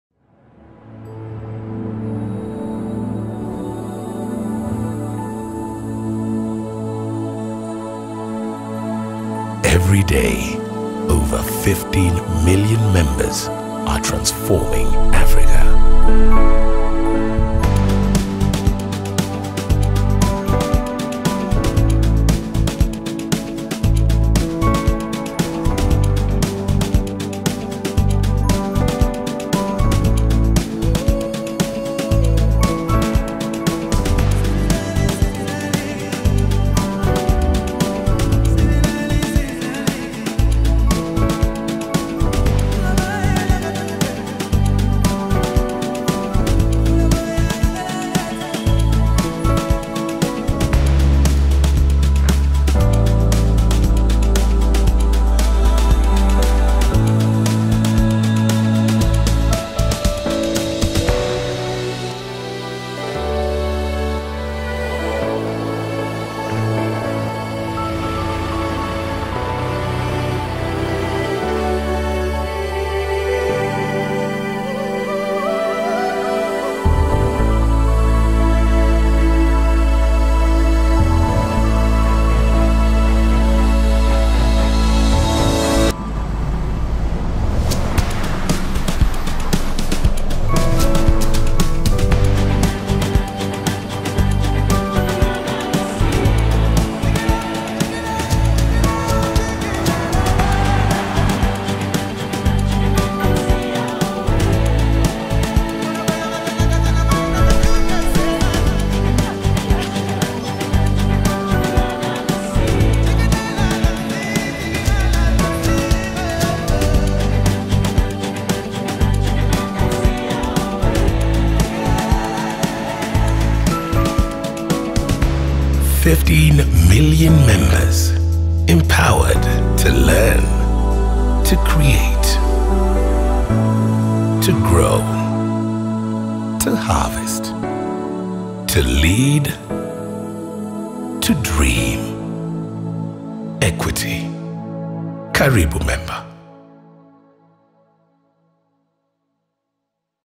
Celebrating over 15MillionMembers Transforming Africa
Today we celebrate over 15MillionMembers empowered across 7 African markets to Learn, Grow, Dream, Create, Lead and Harvest. This is our One Equity spirit!